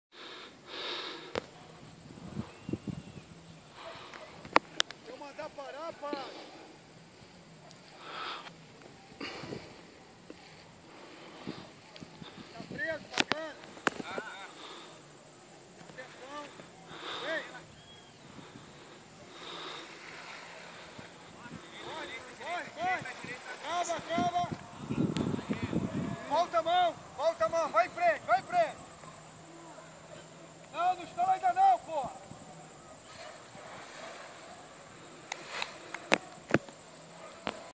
Primeiro voo do Jeffinho
Primeiro voo solo do aluno Jeffinho no Curso de parapente Afonso Urbieta em Geneciano, Nova Iguaçu/RJ